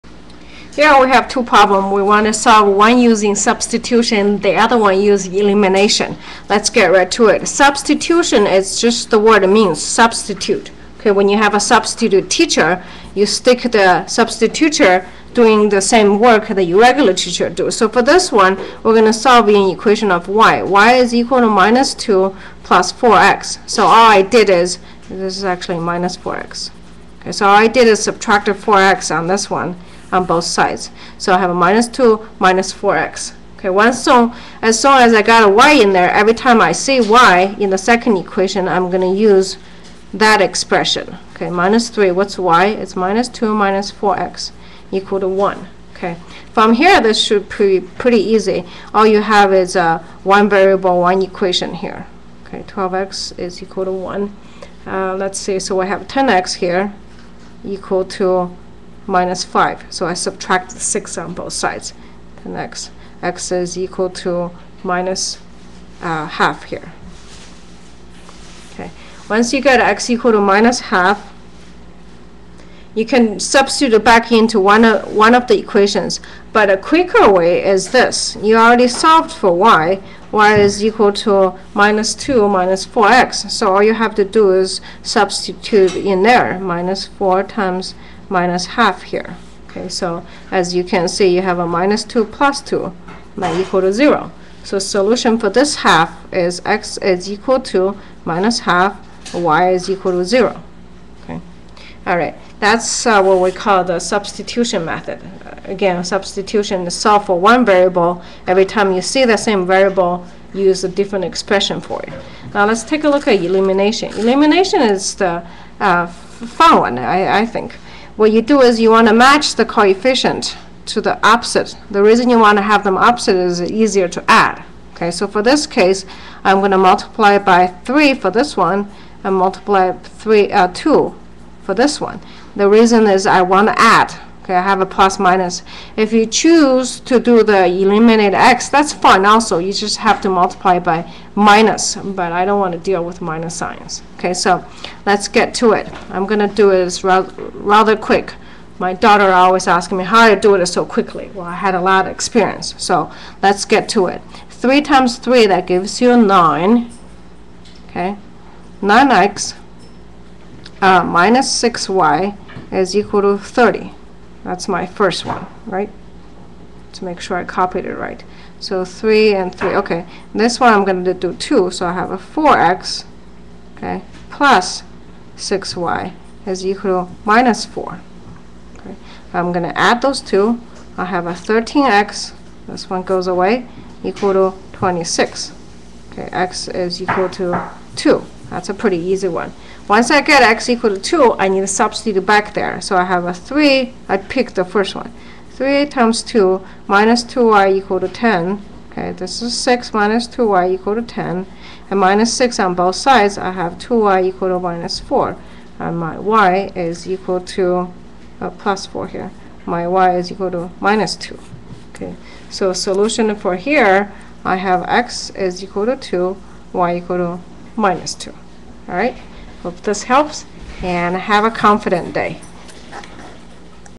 ^*System of Equations with 2 VAR: elimination and substitution
A production. Dr. Pan makes learning math Fun.
Solve by substitution
4x + y = -2
-2x - 3y =1
Solve by elimination
3x - 2y = 10
2x + 3y = -2